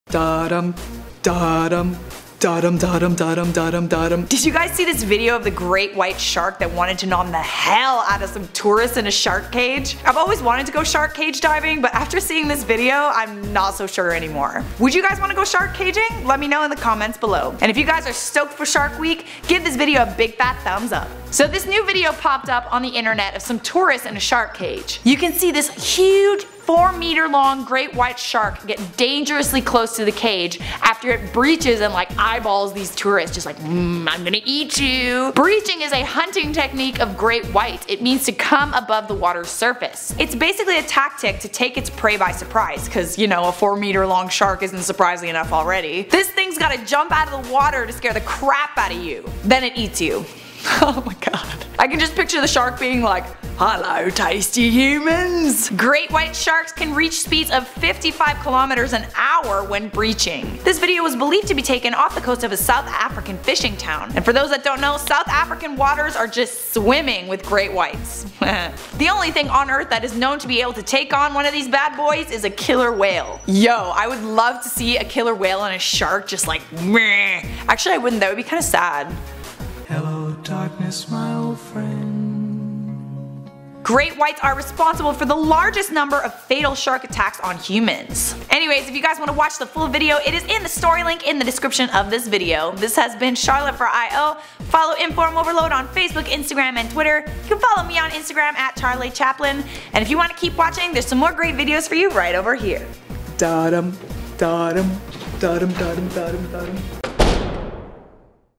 Great White Shark Almost Eats Man While He Was Filming
A group of divers got dangerously close to the face of a great white shark. What was once a fun filled diving excursion turned into a fear filled shark attack and we've got all the details here for you on iO!

2408 Lakeshore Blvd W.